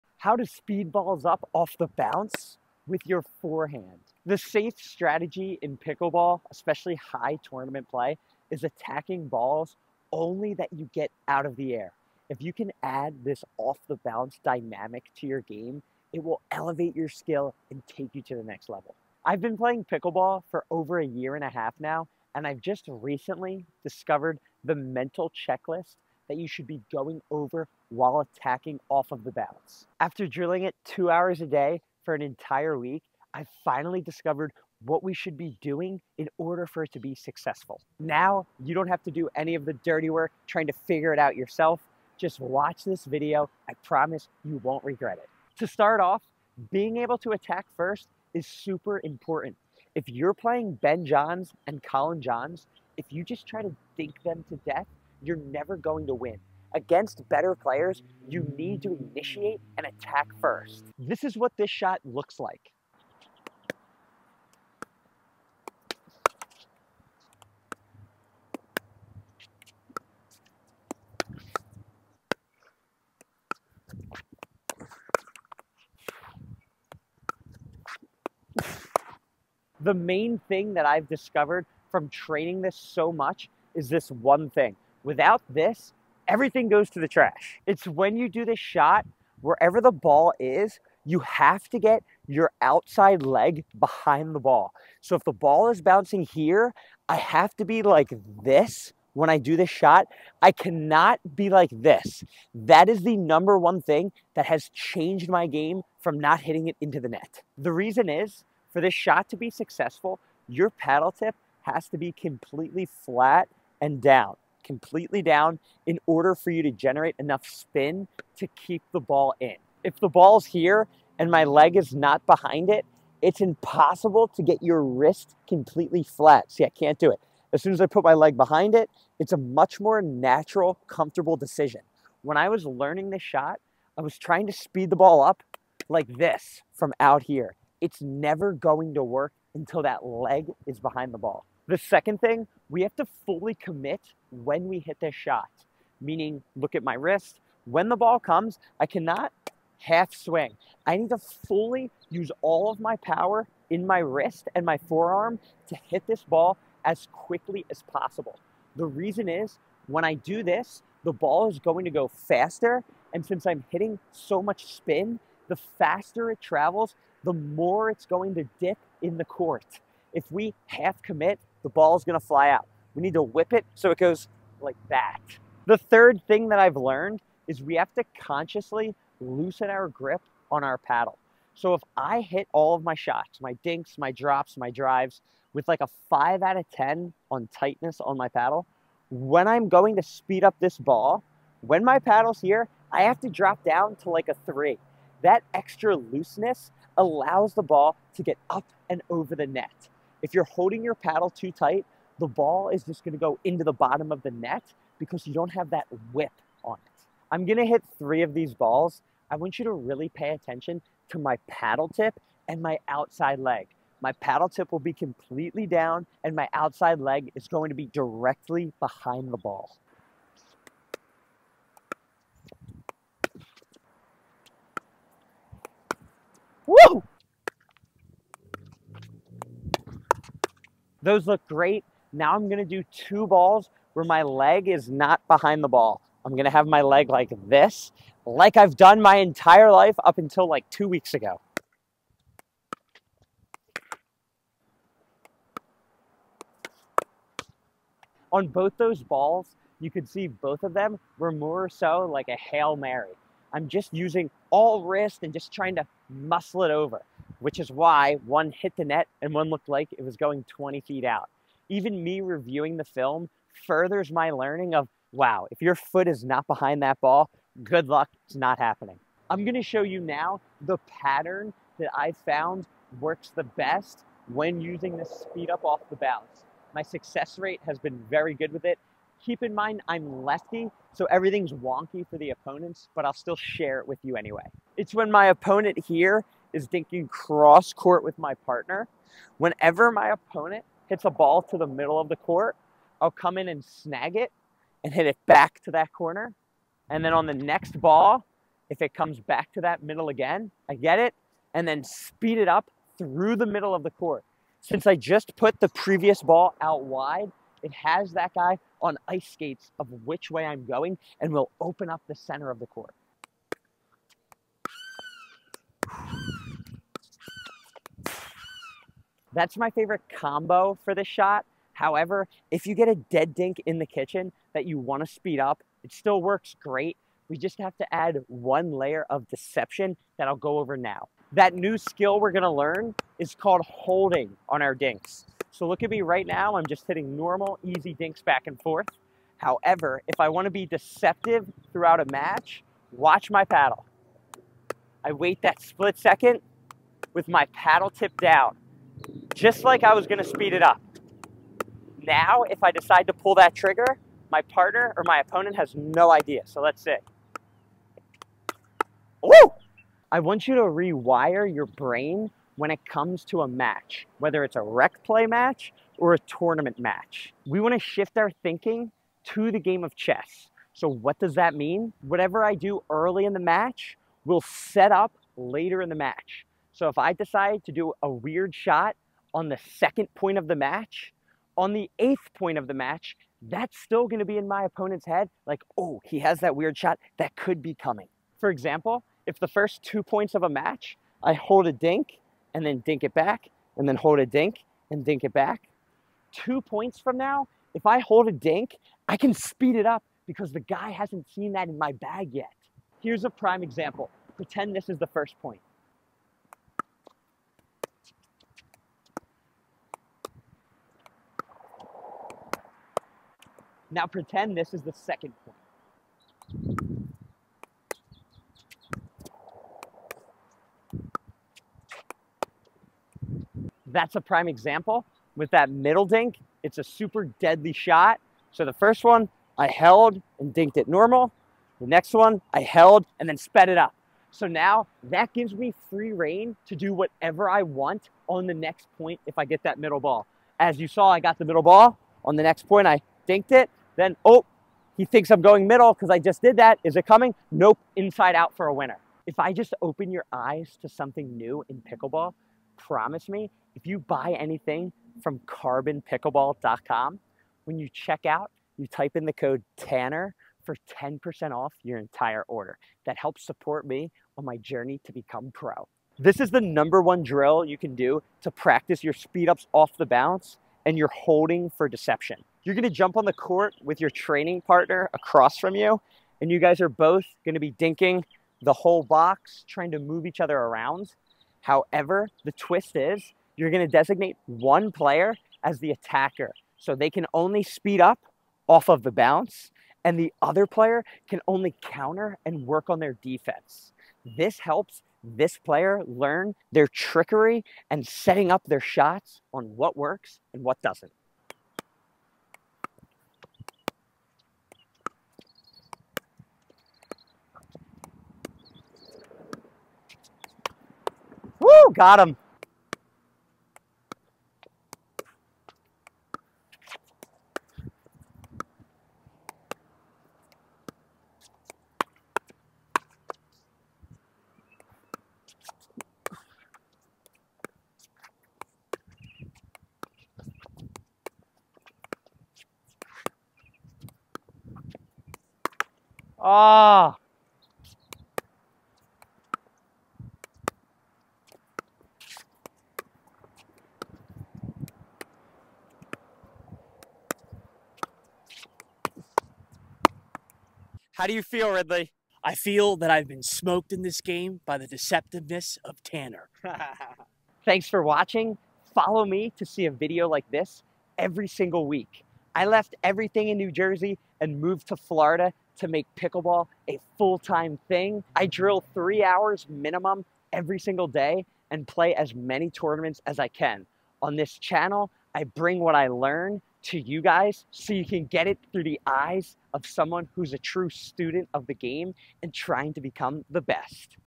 I Hit This Speed Up 1,000 times.. Here's What I Learned
If you were to face off against Ben Johns, you will NEVER win by dinking him to death.. ☠️❌

You HAVE to be first to strike in order to have a chance in the match..🐍

Mastering the off the bounce speedup will set you up to win SO many points if executed properly. 🏆🥇

My biggest takeaway is.. you MUST have your outside leg behind the ball in order for this shot to work. 🧠💯

If you're caught reaching for it, the ball will end up in the net..😔😭

 Having the ball close to your body will allow you to get the paddle tip completely down, generating the most amount of topspin. 👍

Timestamps
0:00 Intro
1:00 Off The Bounce Attack
1:19 KEY things to know for this to work
4:29 Most Successful Pattern
5:19 Adding Deception
7:46 The Best Drill to Practice This
9":17 Outro

💪 Use the same gear as me! 💪
Lead Tape (for paddle)
My Shoes
My Shoes (for women)
Dura Fast 40 Balls